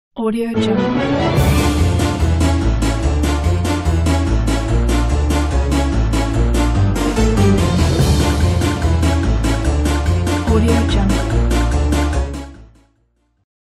Halloween 👻 (After Effects template + Halloween music 🎃 )
-No extra plug ins.
-Control panel for one click edit.
-100% After Effects.
-Video tutorial included
-Links for the free font included.